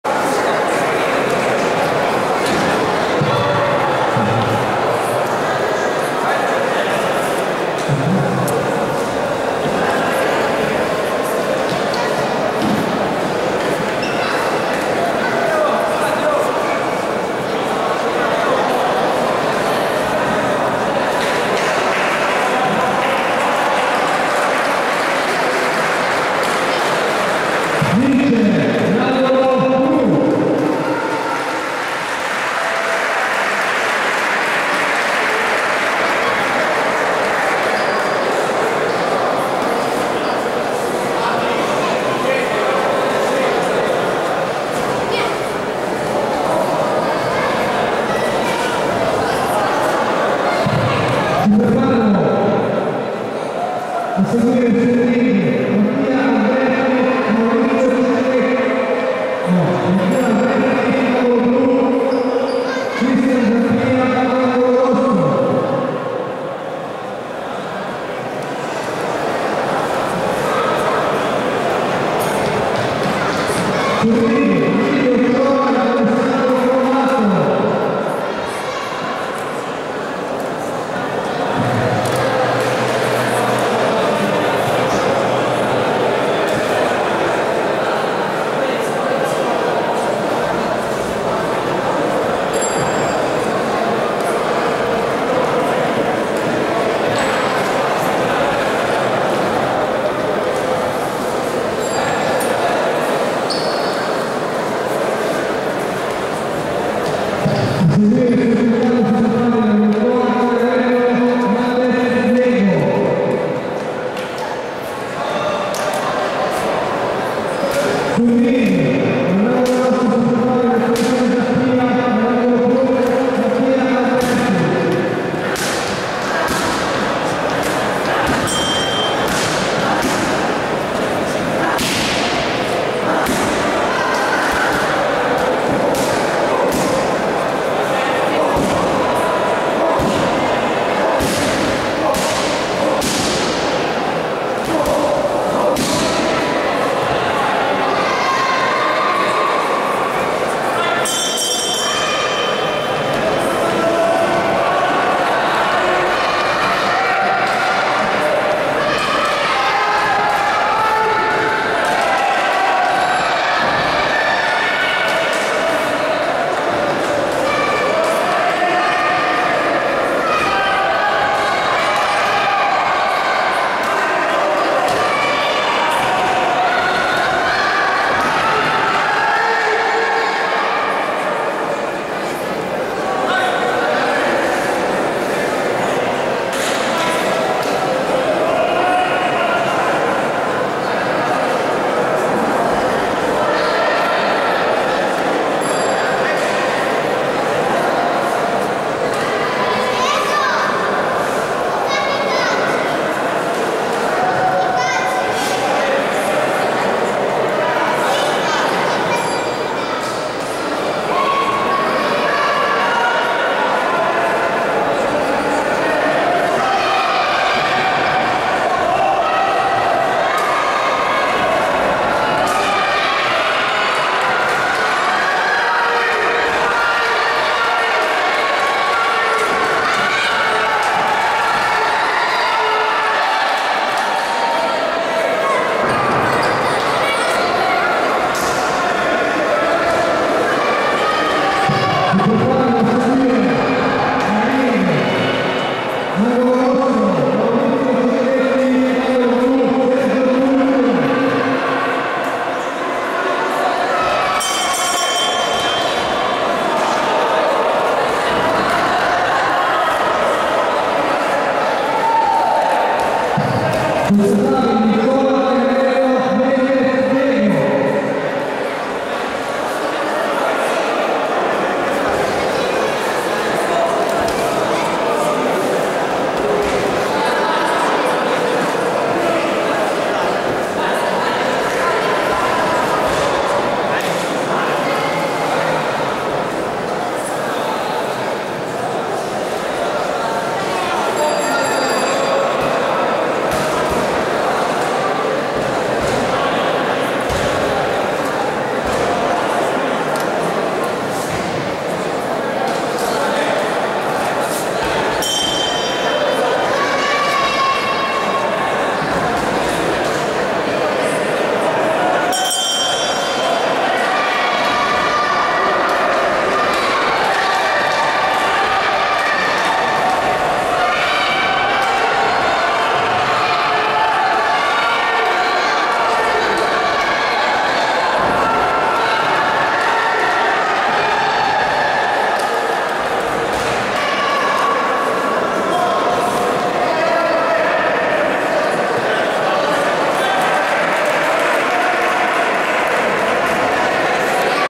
Reactive Club - Manuela Calzolari K1 Light - 14/12/14 Leini (TO)
Incontro di K1 light degli atleti della palestra Reactive Club presso il palazzetto sportivo di Leini (prov. Torino - Turin).
- Organizzazione Top Kombat Confederation -